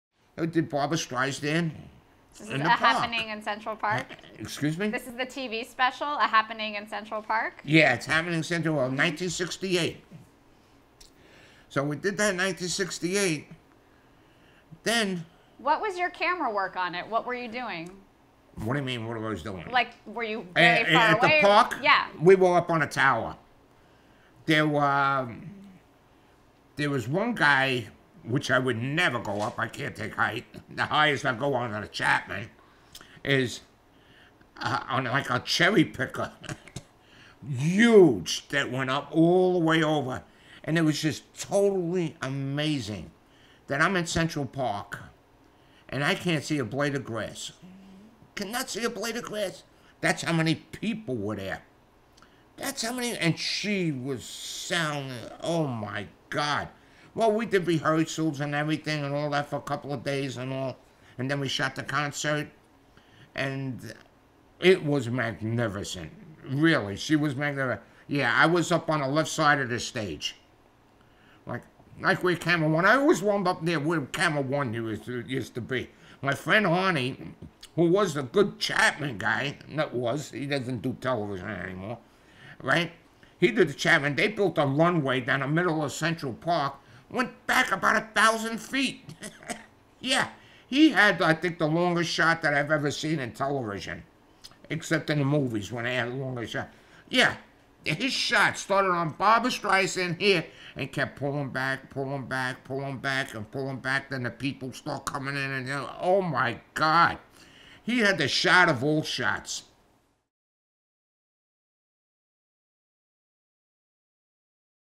Camera Operator Frank Biondo on A Happening in Central Park - TelevisionAcademy.com/Interviews
All content copyright The Television Academy Foundation Interviews.